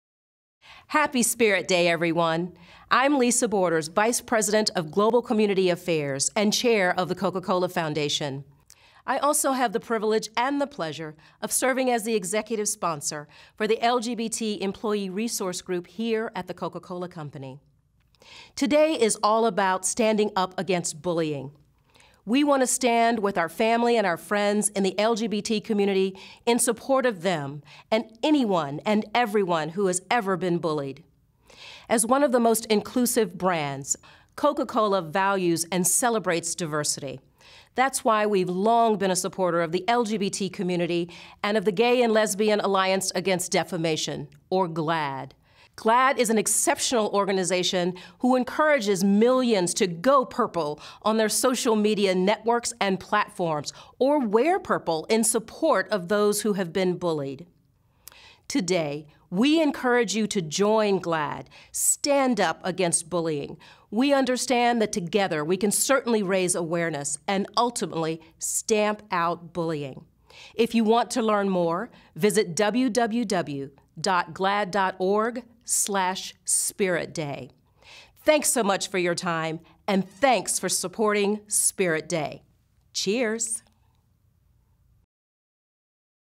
Coke’s Lisa Borders Explains How You Can Support Spirit Day
To help spread the word against bullying, Lisa Borders, VP Global Community Connections and Chair of The Coca-Cola Foundation, explains why it’s important take a stand against bullying on Spirit Day – a day where millions of people “go purple” to show their support for LGBT youth. In addition to Lisa’s responsibilities with our foundation, she also serves as the executive sponsor for our LGBTA employee resource group.

Spirit Day began in 2010 as a way to show support for LGBT youth and encourages people to take a stand against bullying. To learn how you can support Spirit Day, please visit GLAAD’s Spirit Day